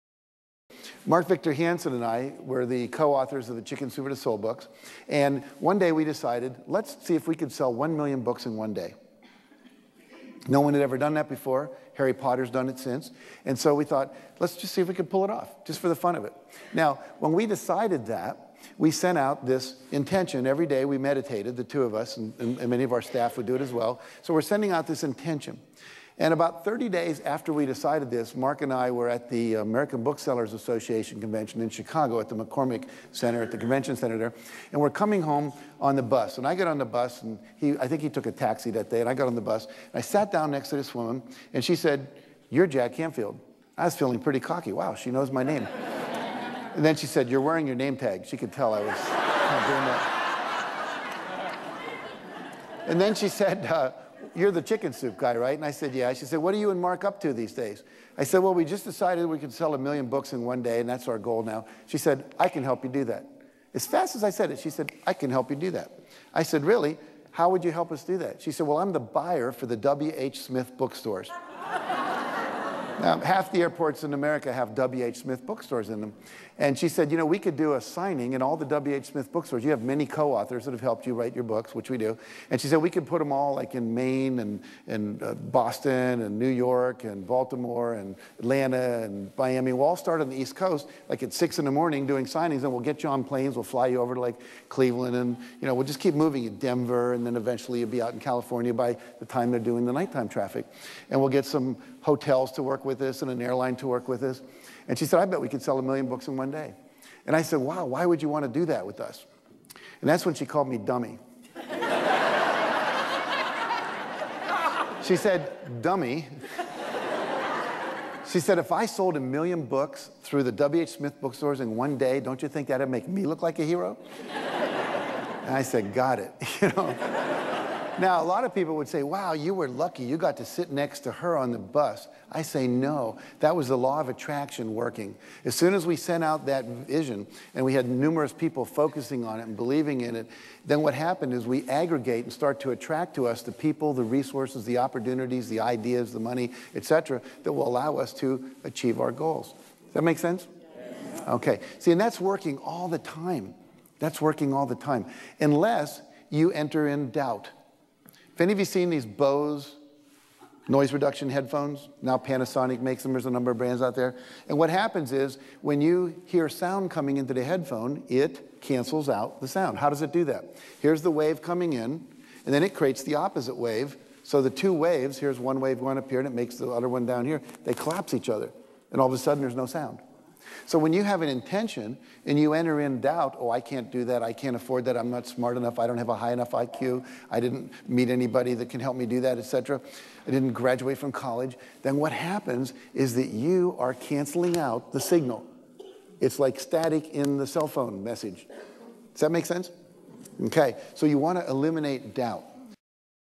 Dream Big: Jack Canfield on Turbo-Charging Results
It takes just as much focus and mental effort to achieve a small goal as it does a big, gargantuan one  so what are you doing wasting your time on piddly achievements? Youve got BIG dreams in your head  so step up to those BIG dreams and start acting on them, even if youre not exactly sure how its all going to shake out, or even what the next step should be. Secret teacher and Chicken Soup for the Soul Co-Founder Jack Canfield walks you through the process

Buy this DVD as part of Jack Canfields Dream Big System and get a free, 72-minute instant audio bonus